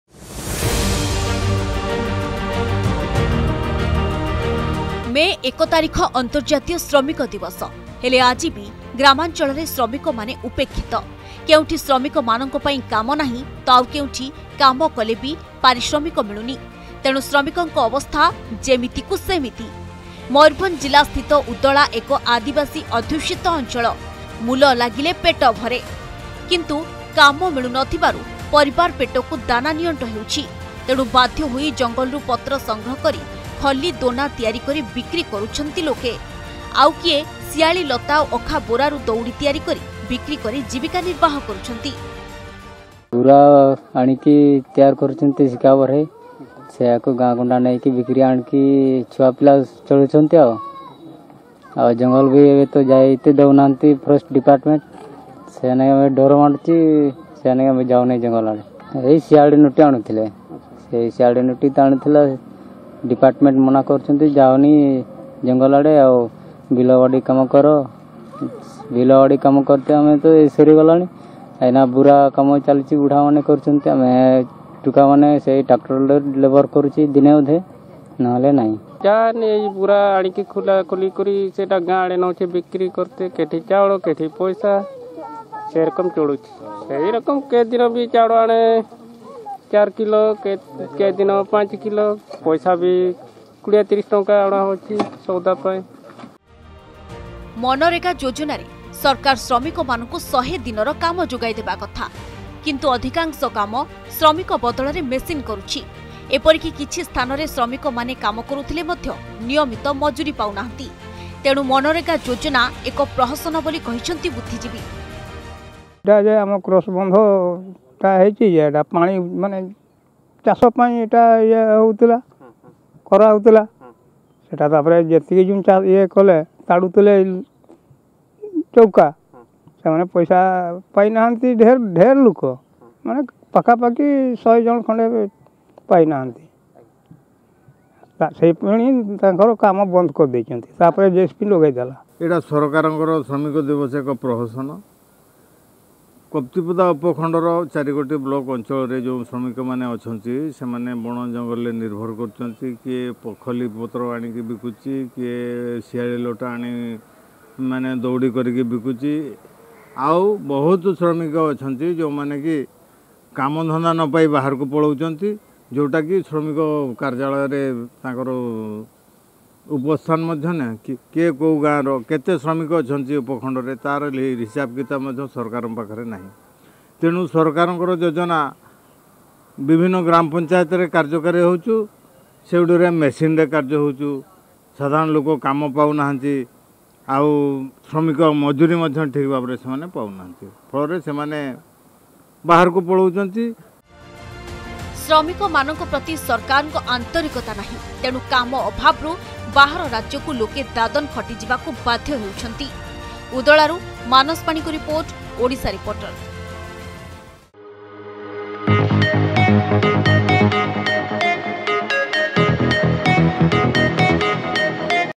ଶ୍ରମିକଙ୍କୁ ମିଳୁଛି ତ ଶ୍ରମ… ? | Odisha Reporter
ମେ ୧ ତାରିଖ ଅନ୍ତର୍ଜାତୀୟ ଶ୍ରମିକ ଦିବସ । ହେଲେ ଆଜିବି ଗ୍ରାମାଞ୍ଚଳରେ ଶ୍ରମିକ ମାନେ ଉପେକ୍ଷିତ। କେଉଁଠି ଶ୍ରମିକ ମାନଙ୍କ ପାଇଁ କାମ ନାହିଁ, ତ ଆଉ କେଉଁଠି କାମକଲେ ବି ପାରିଶ୍ରମିକ ମିଳୁନି। ତେଣୁ ଶ୍ରମିକଙ୍କ ଅବସ୍ଥା ଯେମିତିକୁ ସେମିତି। ମୟୂରଭଞ୍ଜ ଜିଲ୍ଲା ସ୍ଥିତ ଉଦଳା ଏକ ଆଦିବାସୀ ଅଧ୍ୟୁଷିତ ଅଞ୍ଚଳ। ମୂଲ ଲାଗିଲେ ପେଟ ଭରେ। କିନ୍ତୁ କାମ ମିଳୁ ନଥିବାରୁ ପରିବାର ପେଟକୁ ଦାନା ନିଅଣ୍ଟ ହେଉଛି ।